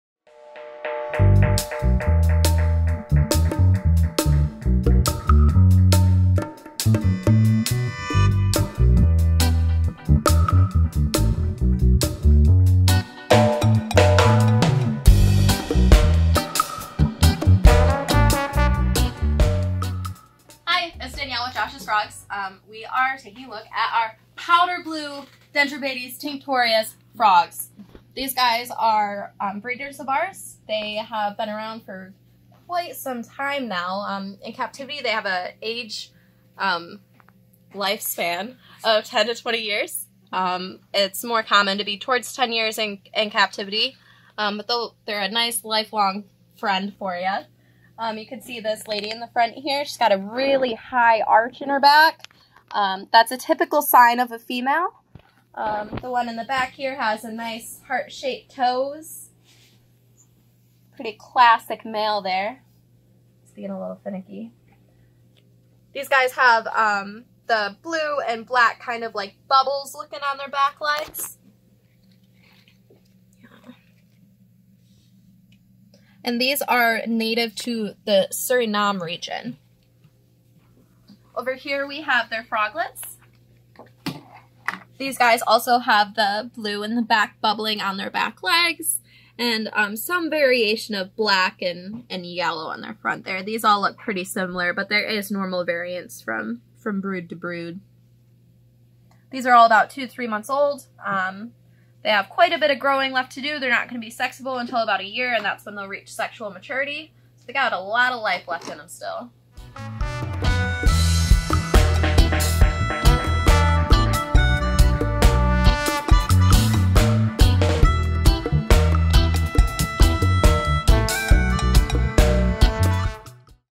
Species Spotlight: Dendrobates tinctorius 'Powder Blue'
Brilliant!! The color of these frogs couldn't be more satisfying,  Dani has a ton of facts to help curb your curiosity. If you have any more questions for Dani or just want to say hi, leave us a message in the comments below.

Dendrobates tinctorius 'Powder Blue'.
 Some individuals of this population which had more gray legs were once (and sometimes still are) referred to as Dendrobates tinctorius 'Powder Gray'. In the hobby, these poison dart frogs are commonly called powder blue tincs, or just powder blues or powders.